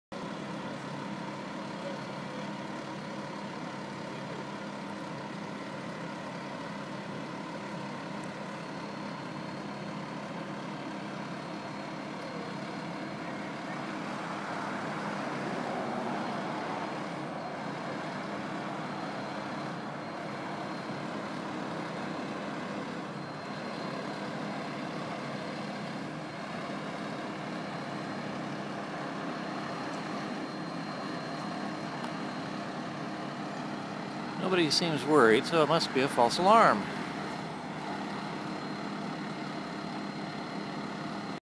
A moment's excitement
Visalia Fire Dept. responds to a false alarm.